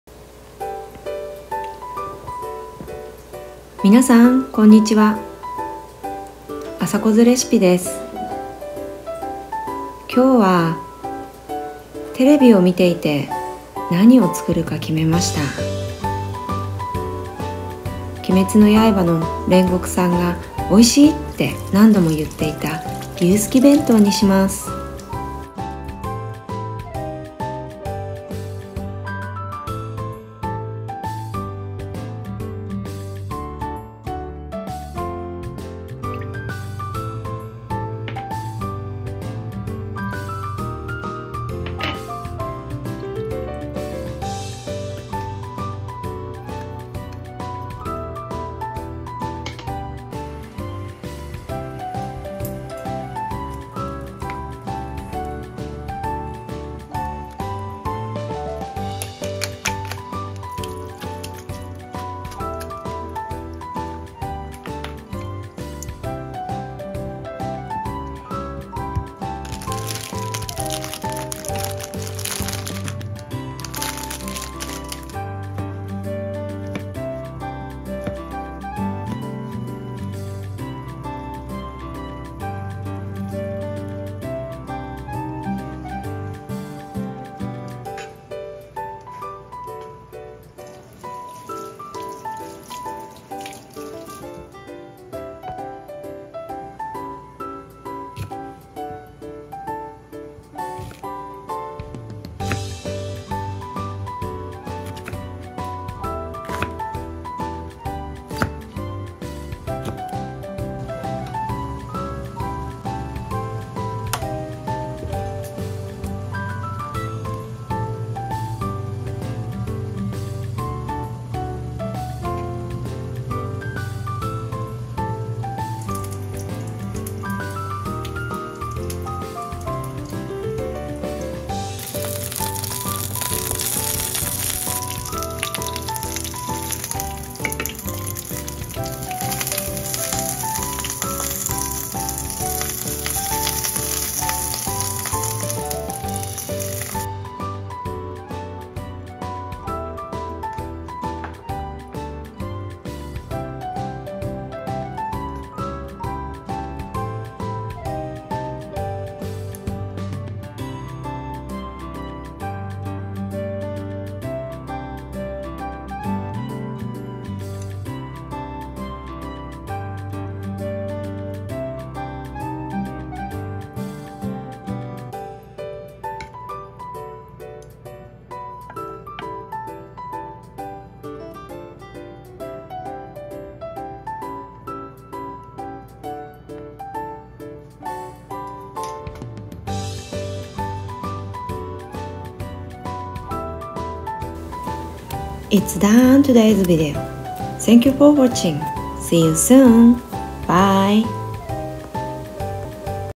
Recreating the bento from Demon Slayer.Cooking is Simple but delicious.cooking time only3min!
welcome to my video! This time, I will introduce the sukiyaki bento that appears in my favorite anime, Demon Slayer: Kimetsu no Yaiba. The character, Rengoku-san, repeatedly called out how delicious it was. It tastes convincing when you eat it
Sukiyaki is a luxury food. However, even if it's expensive, it's delicious enough to make you want to eat it. Adding onions to the meat is also delicious.